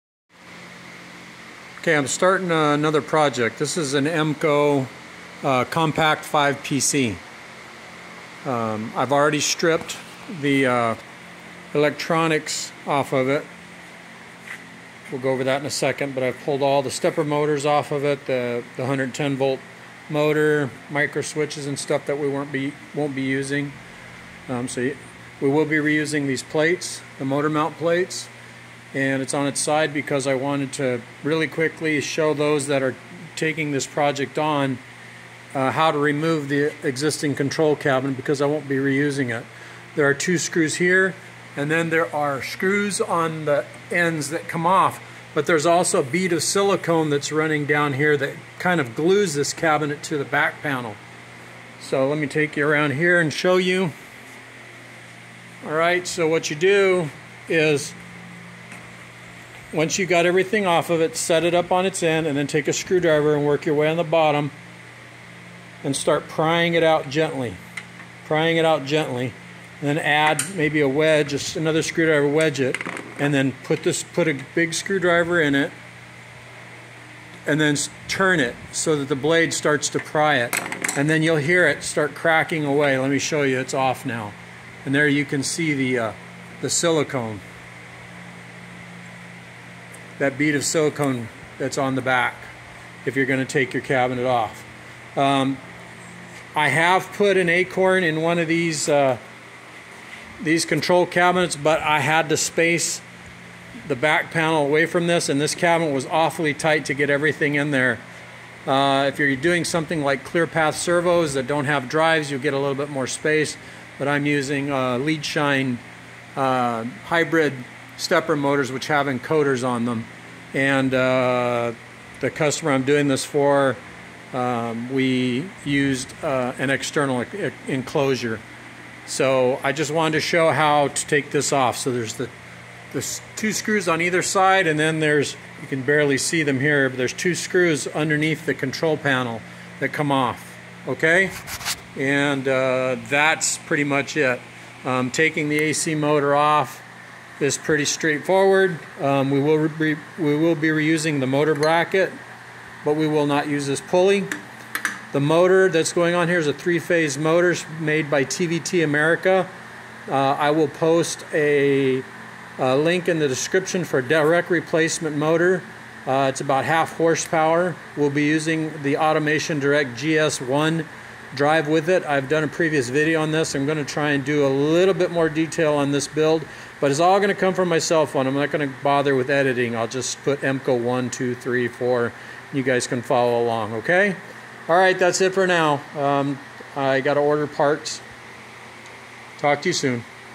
New Centroid CNC Acorn Conversion. Another Emco PC5 Part 1
Part 1 Teardown. How to remove the old cabinet.

I will post parts list and sources as I go. This is only a suggested build.